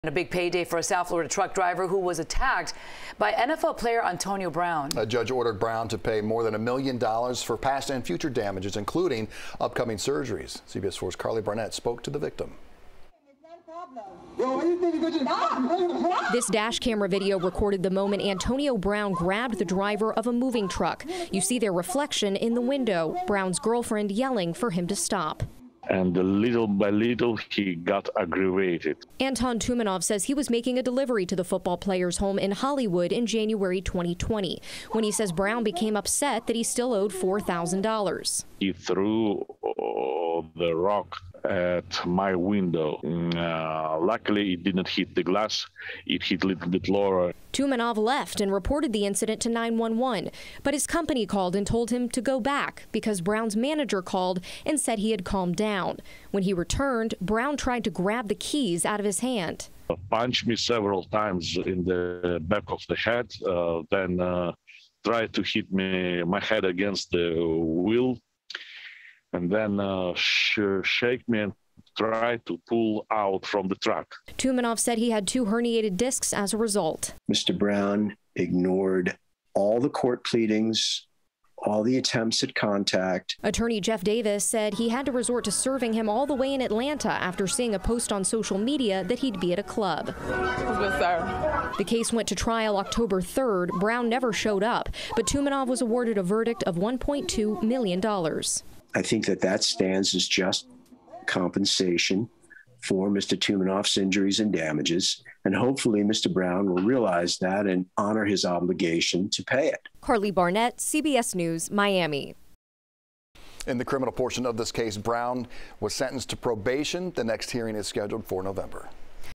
Antonio Brown Ordered To Pay Over $1 Million For Alleged Attack On Moving Truck Driver In Hollywood
Antonio Brown has been ordered to pay more than $1 million to a moving truck driver in Florida after allegedly fighting the man in 2020.